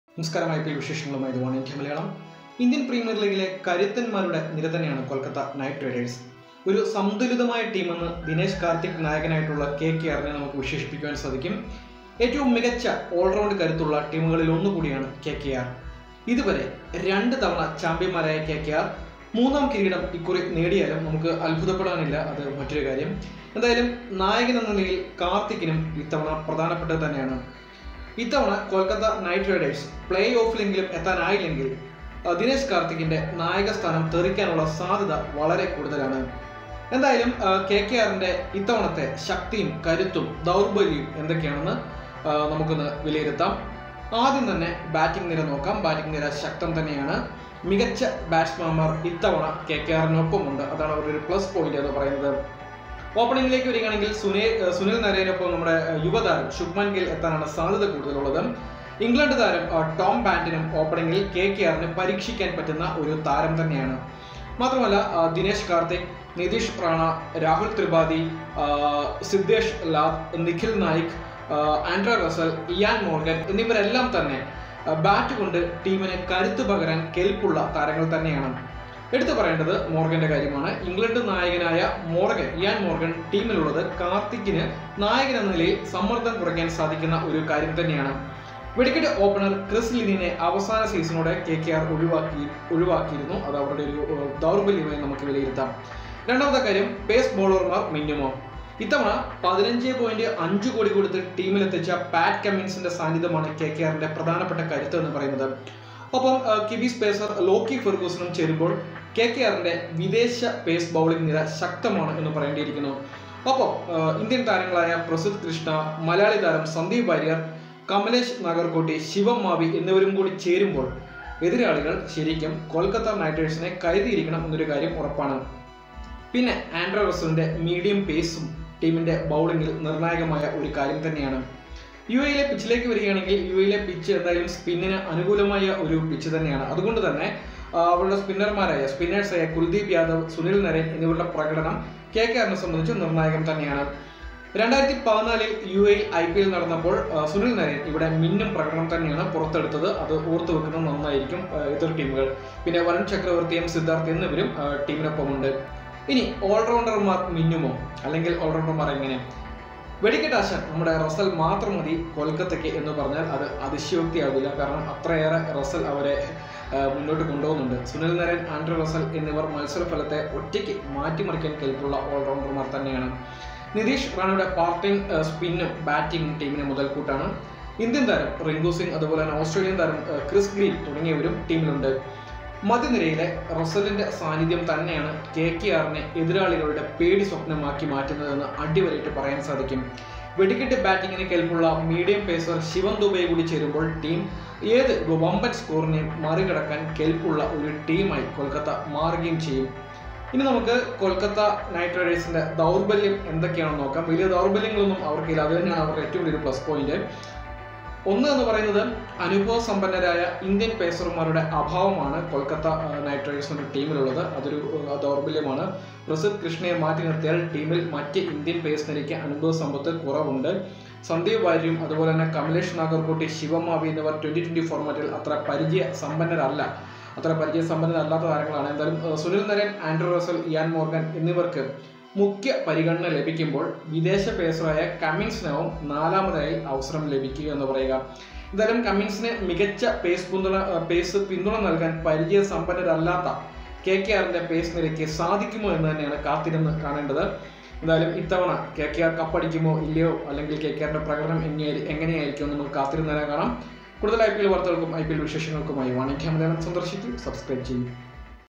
IPL 2020 : Strength and Weakness Of KKR | Oneindia Malayalam
IPL 2020 : Pro and cons of KKR lead by Dinesh Karthik
ഇന്ത്യന്‍ പ്രീമിയര്‍ ലീഗിലെ കരുത്തന്‍മാരുടെ നിര തന്നെയാണ് കൊല്‍ക്കത്ത നൈറ്റ് റൈഡേഴ്‌സ്. സംതുലിതമായ ടീമെന്ന് ദിനേഷ് കാര്‍ത്തിക് നായകനായുള്ള കെകെആറിനെ വിശേഷിപ്പിക്കാം. ഏറ്റവും മികച്ച ഓള്‍റൗണ്ട് കരുത്തുള്ള ടീമുകളിലൊന്നാണ് കെകെആര്‍. ഇതുവരെ രണ്ട് തവണ ചാമ്പ്യന്മാരായ കെകെആര്‍ മൂന്നാം കിരീടം ഇക്കുറി നേടിയാലും അത്ഭുതപ്പെടാനില്ല.  കെകെആറിന്റെ ഇത്തവണത്തെ കരുത്തും ദൗര്‍ബല്യവും വിലയിരുത്താം.